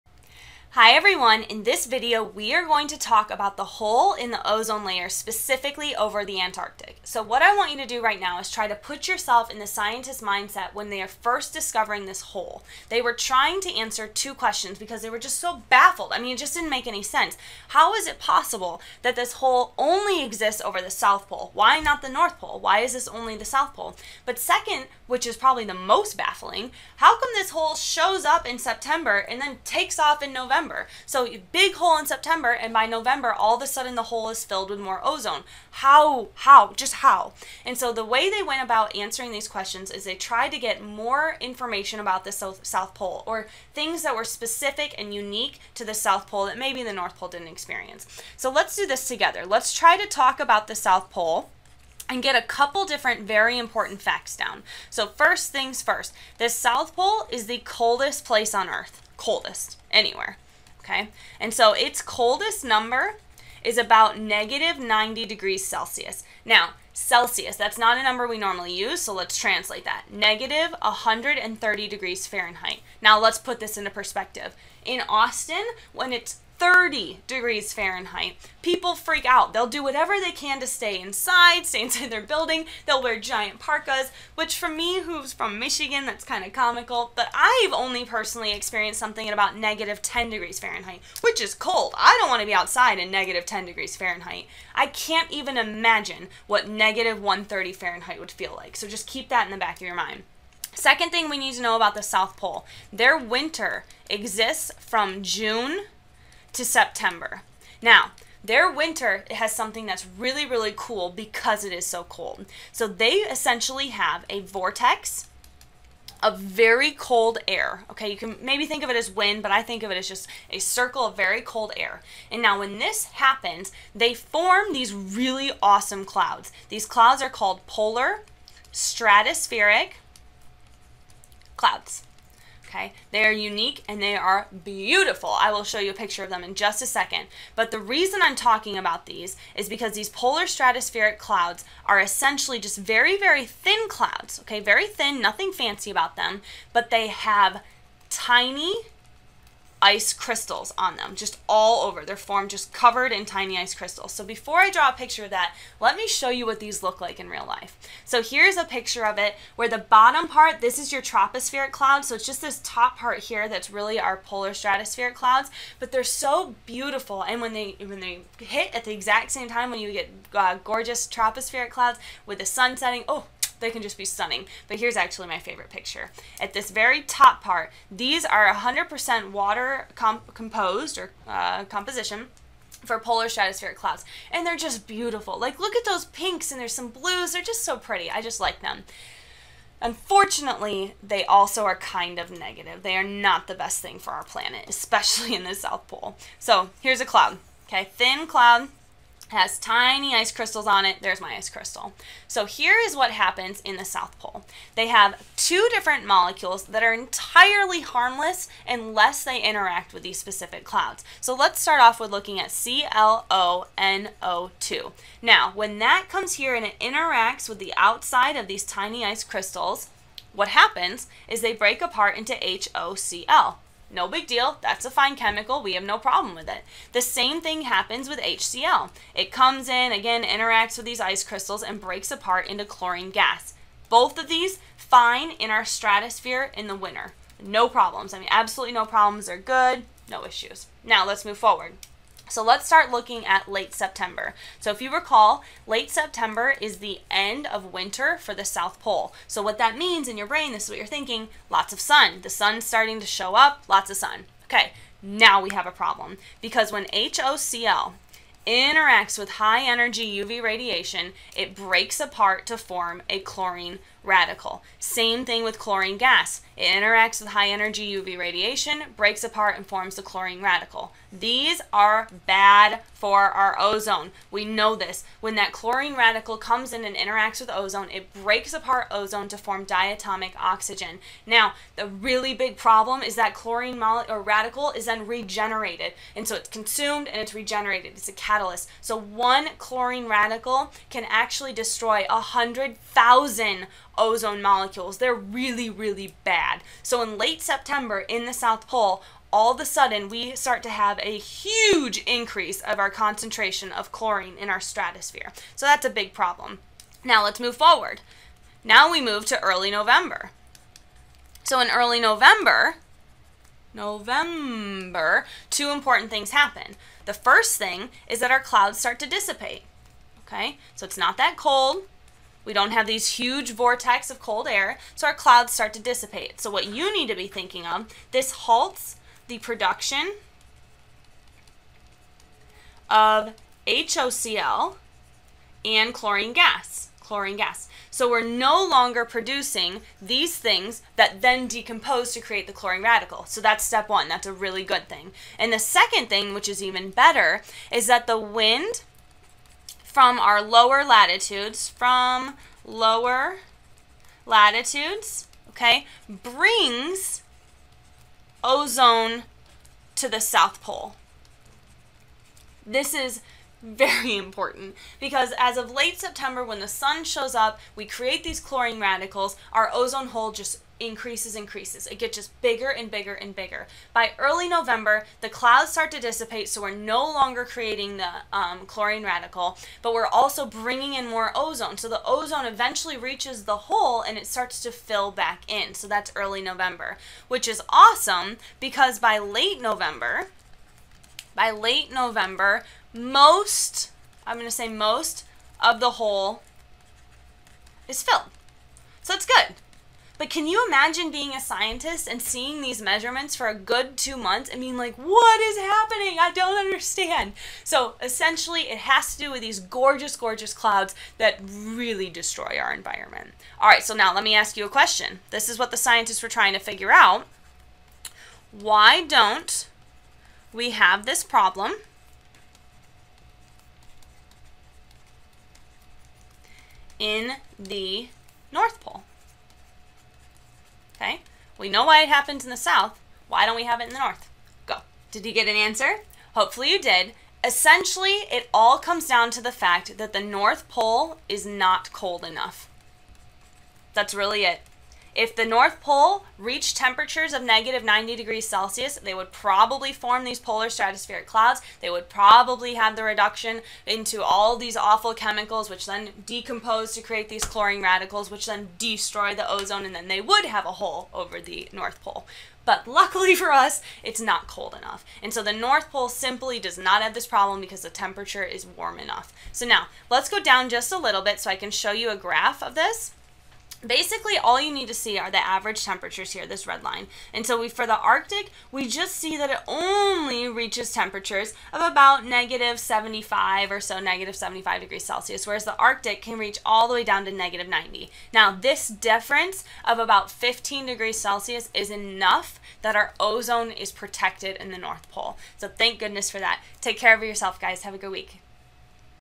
Antarctic Ozone Hole
A teaching video on the Antarctic Ozone Hole used in 'The Ozone Layer' module at The University of Texas at Austin.